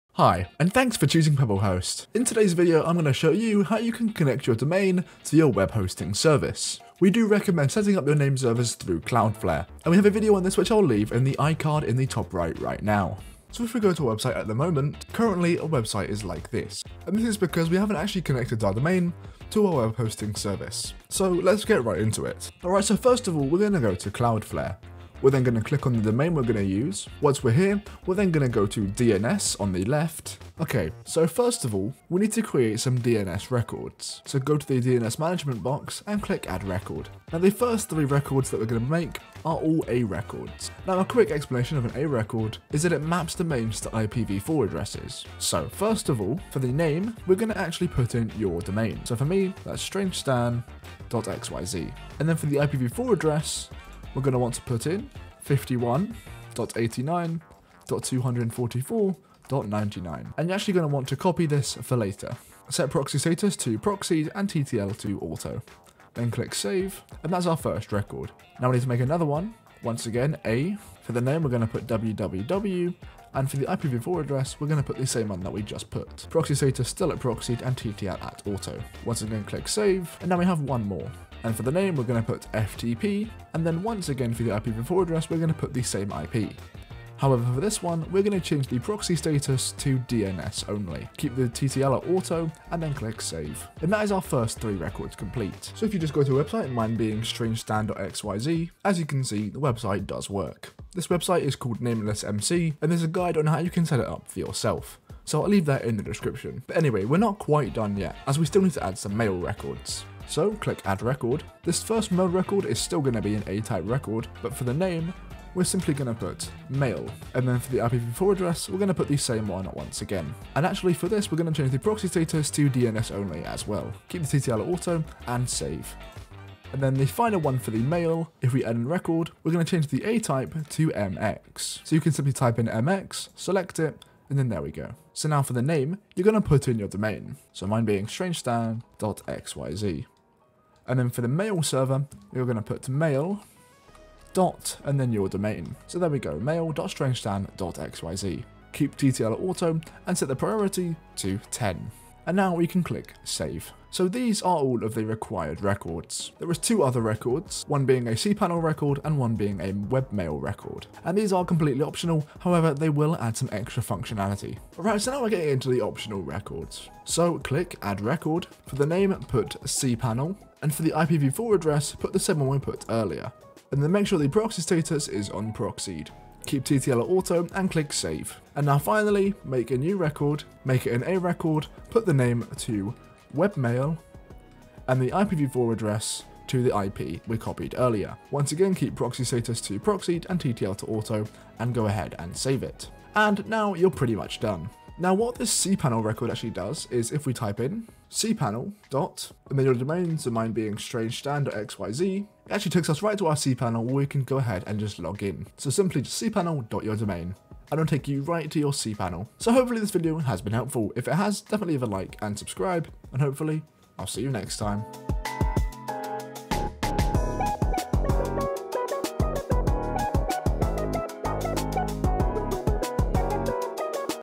How to Connect a Domain to Web Hosting
📗 Chapters:
0:00 Introduction and Information
0:06 Cloudflare Recommendation
0:22 DNS Records (Required)
1:50 Mail Records (Required)
2:44 Extra DNS Records (Not Required)
3:29 Outro